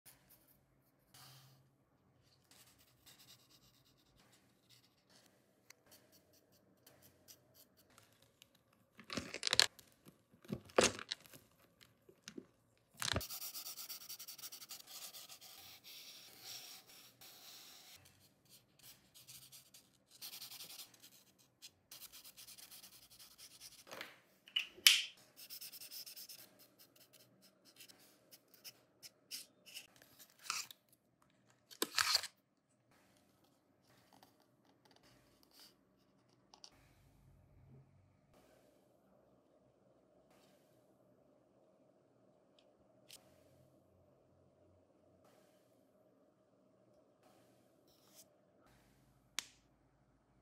❤️ Diana In Red
Continuing with red dresses, today I decided to use the Ralph Lauren dress that Diana wore. I hope you like it❤️